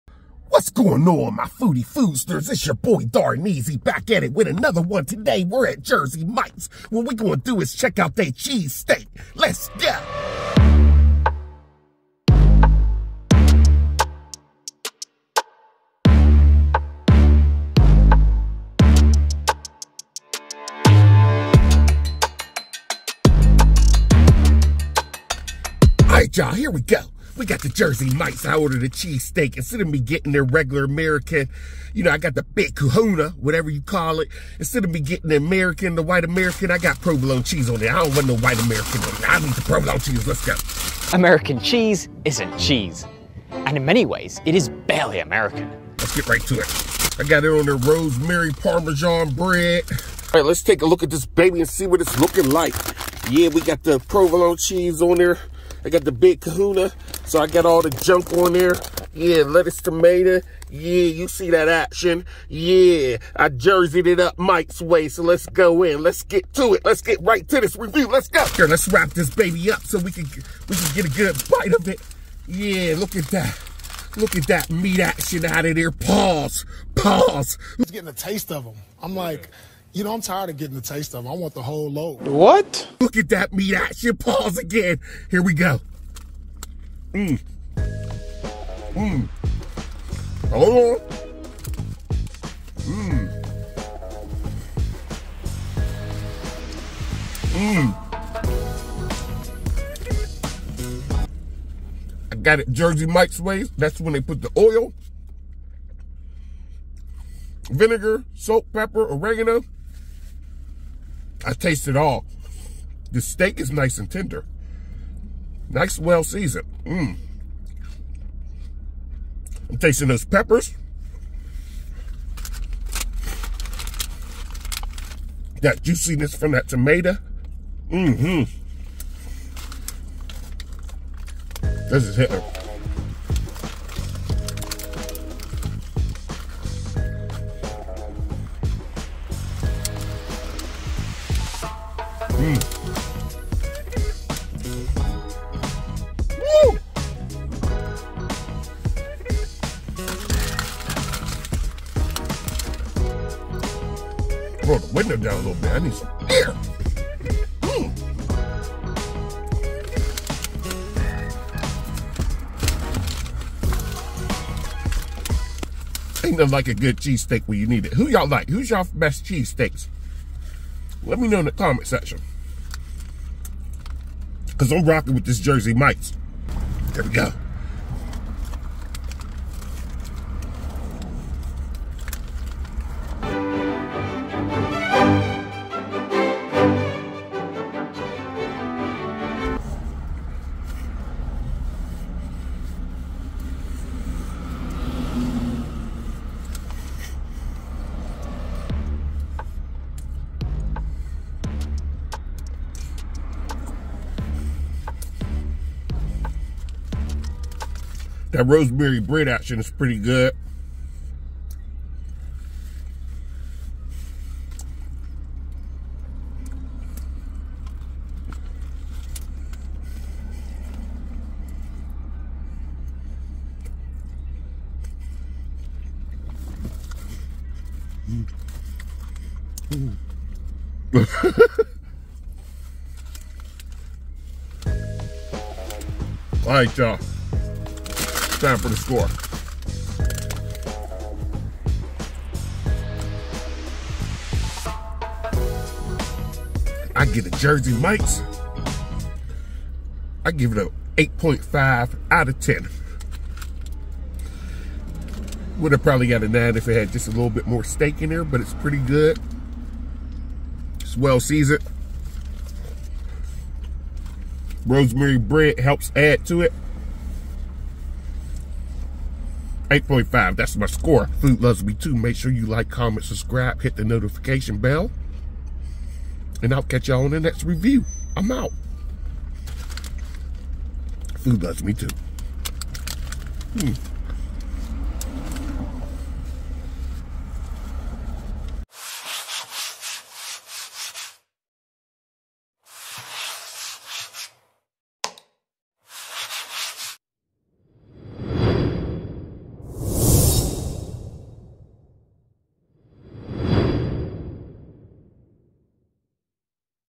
(PAUSE) At Jersey Mike's For The Big Kahuna Cheese Steak review. Is it Better Than Subway?
Trying And Reviewing Jersy Mike's Big Kahuna Cheese Steak